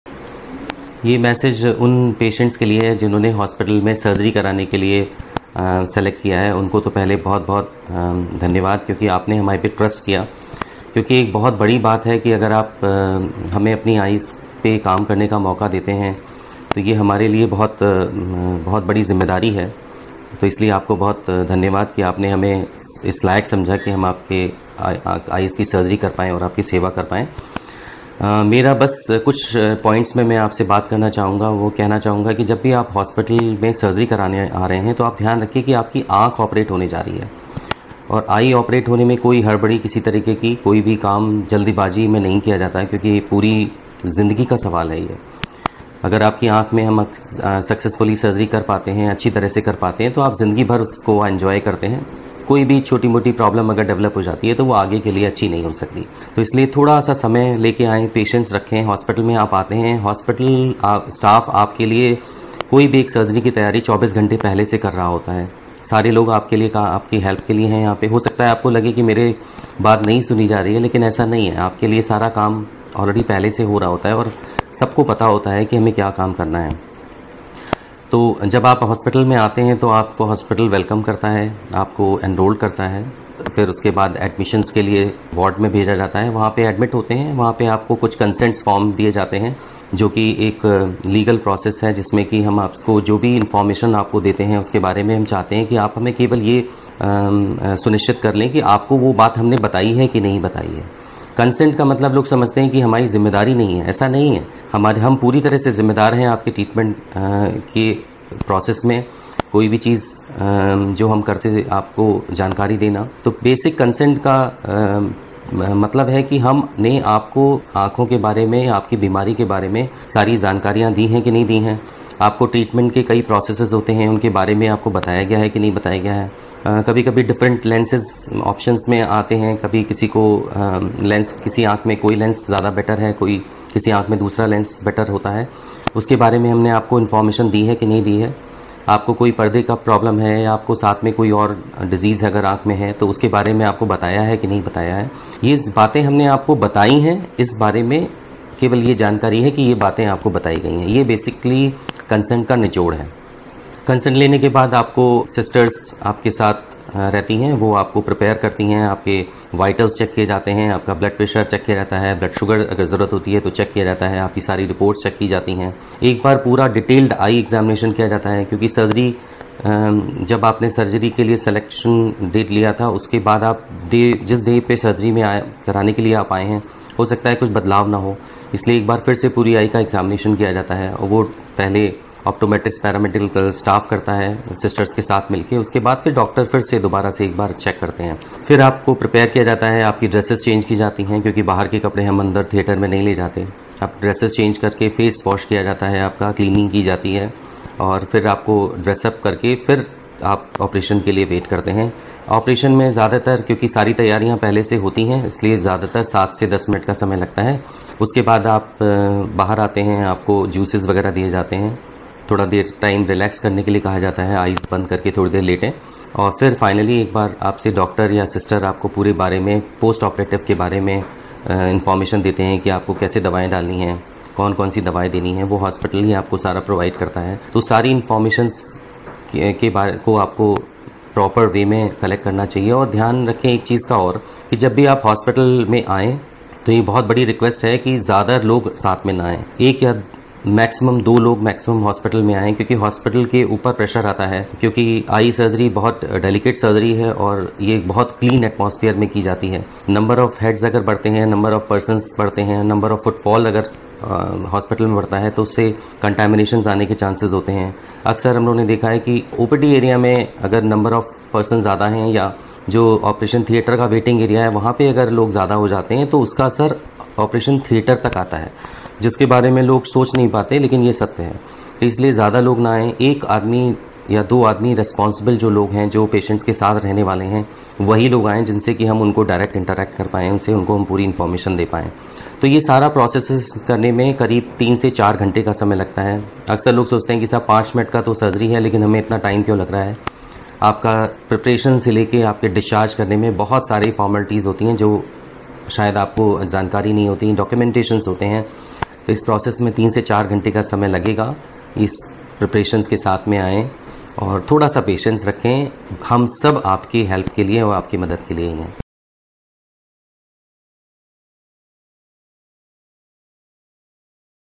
Message to Patients and their attendants who have planned Eye Surgery -(Hindi) Dr Sudhir Srivastava
Message by Dr Sudhir Srivastava for the patients and their attendants who have selected SunEye for their Eye Surgery